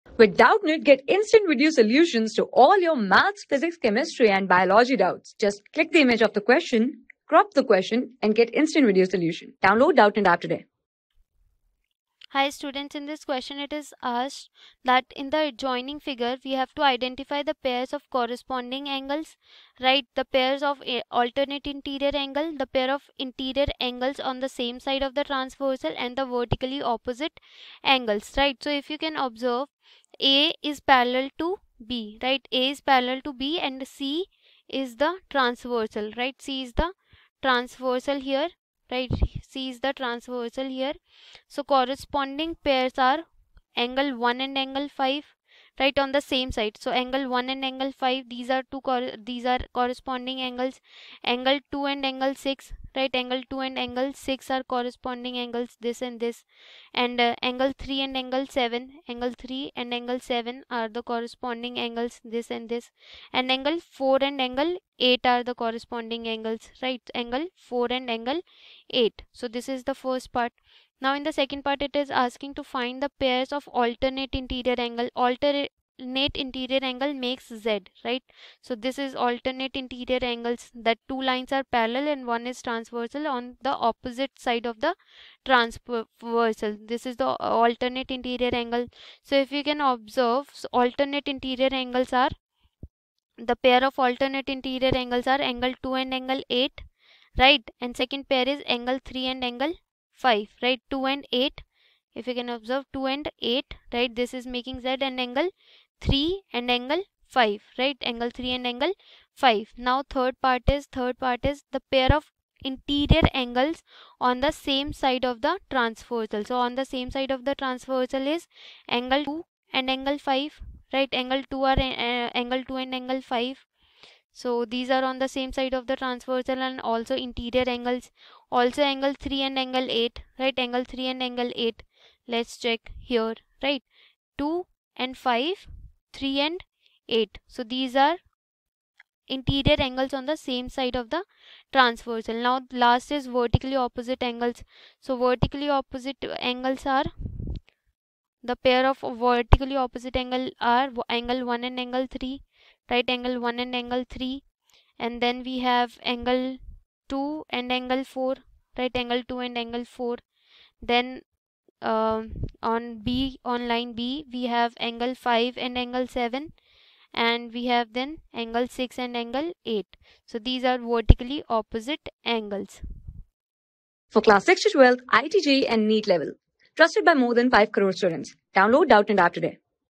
In the adjoining figure, identify(i) the pairs of corresponding angles.(ii) the pairs of alterna...
In the adjoining figure, identify(i) the pairs of corresponding angles.(ii) the pairs of alternate interior angles.(iii) the pairs of interior angles on the sameside of the transversal.(iv) the vertically opposite angles.
Class: 7
Subject: MATHS
Chapter: LINES AND ANGLES
Board:CBSE

👉 Have Any Query? Ask Us.